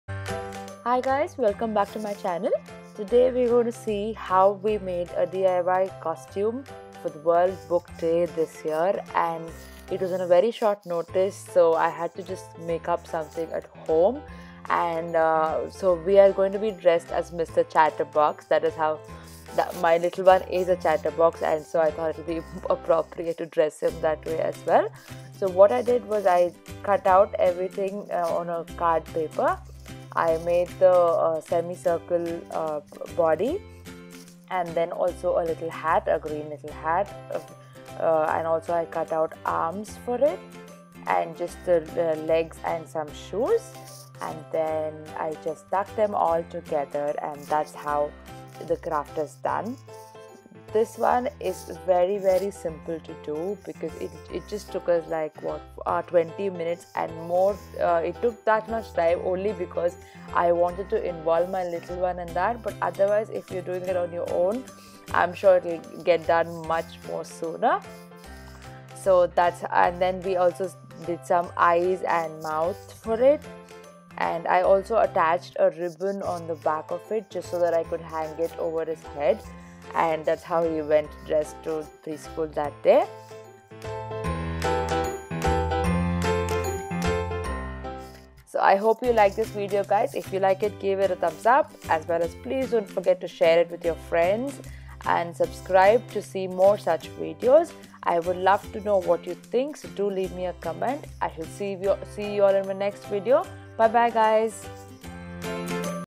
DIY costume Mr. Chatterbox Episode 4
When it is the time to play dress up  , it is also the perfect time to involve your child into making his own costume .This simple costume idea is perfect for beginners and you can't go wrong :)